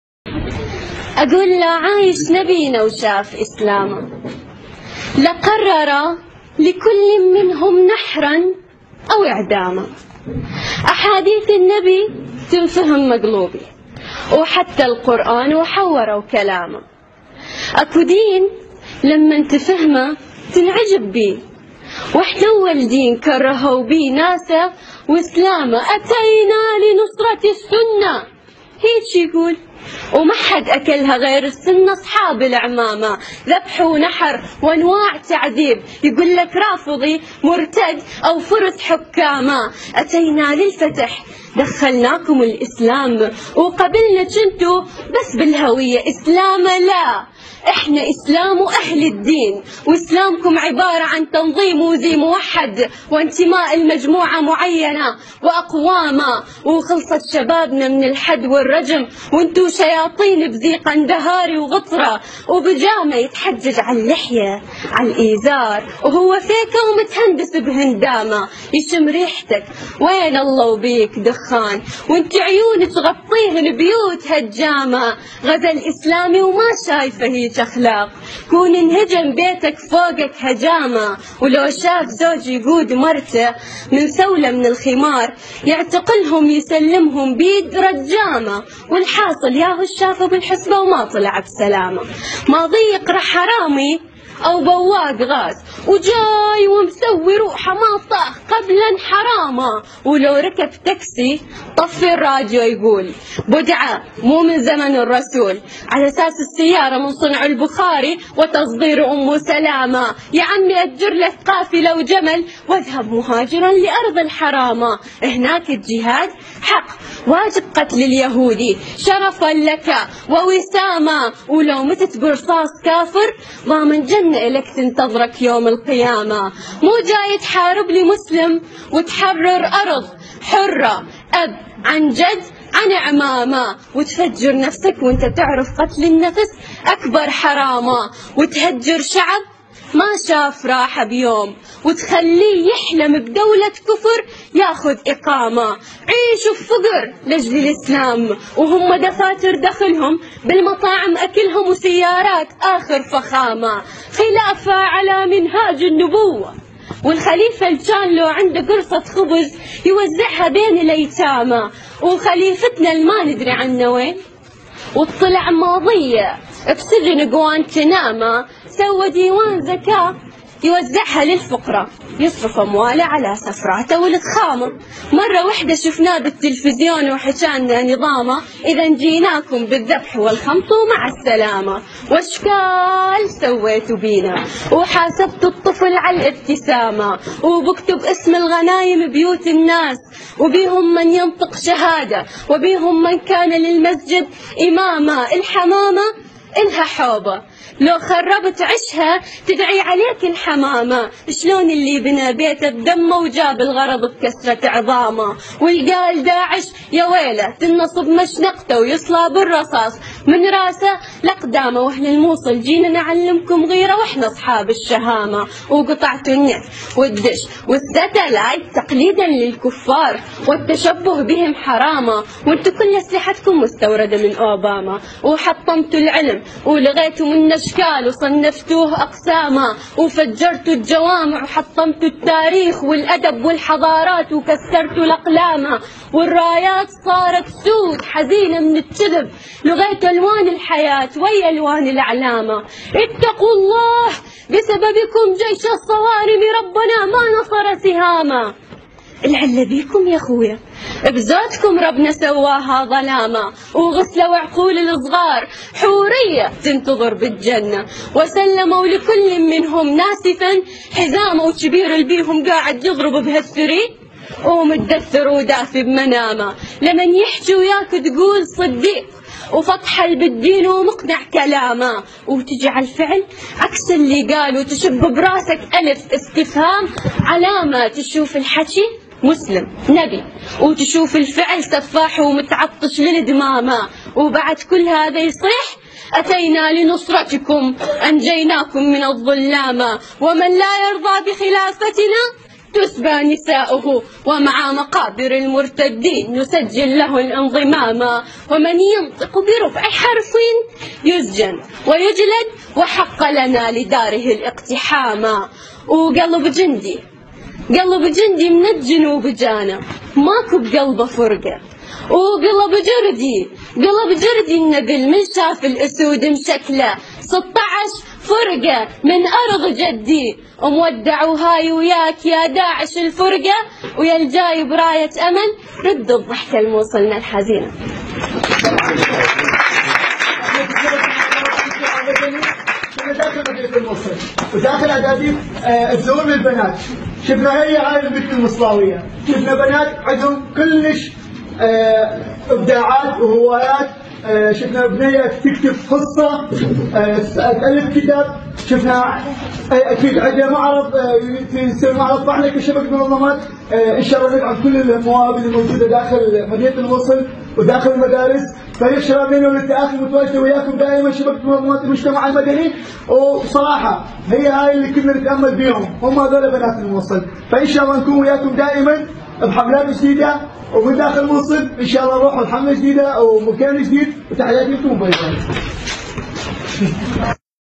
شاعرة الموصل تحرق داعش
بنت موصلية تلقي قصيدة ضد داعش